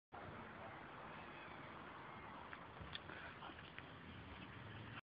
Cubas Funny Little Walk
Our staffordshire Bull Terrier Cuba and the funny little walk she does!